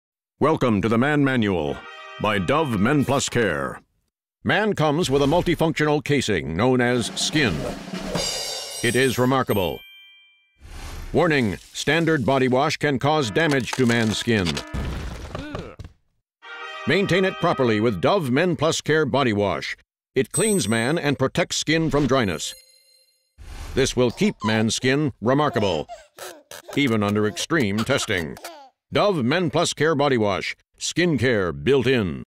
Dove Men+Care 'Protecting Man's Outer Layer'
Director: John Robertson
Production Company: Passion Pictures

I was one of five compositors on this global series of commercials, and was lead compositor on the project for 7 months. This is one of the spots that I comped using After Effects. The characters were created and animated in XSI.

30 secs